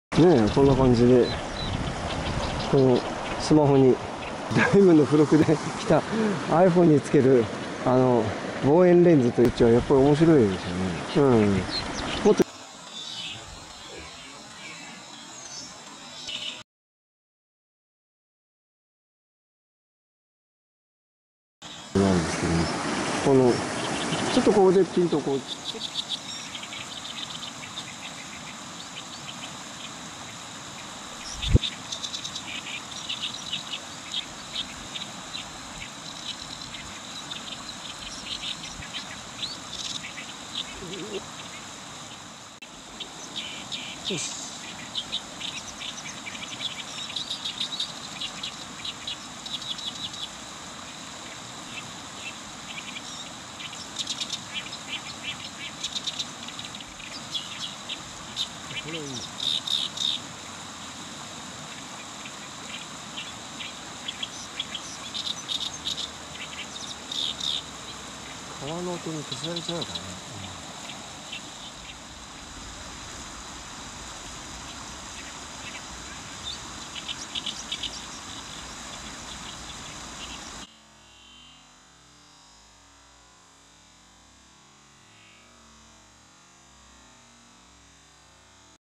軽米町内の雪谷川で野鳥観察をしてみた
雑誌のおまけにスマホ用の望遠レンズがついてきた。
三脚を使ってもブレてピント合わせが大変。
おまけに野鳥の名前をほとんど知らないときた！？
でも観ていたら飽きない。また　鳥を撮り　たいってが